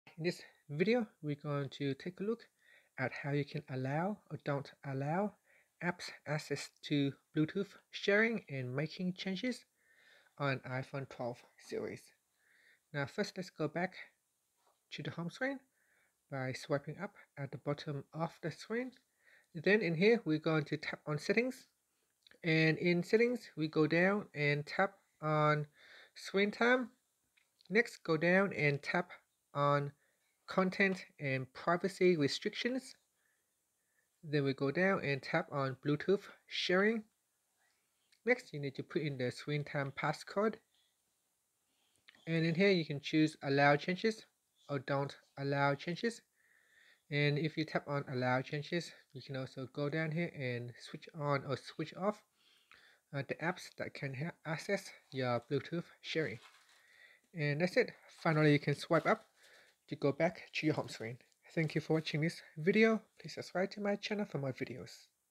iPhone: How to Allow / Don't Allow Apps to Access and Making Changes to Bluetooth Sharing
Learn how you can Allow / Don't Allow apps to access and making changes to Bluetooth Sharing on the iPhone 12, iPhone 12, iPhone Pro.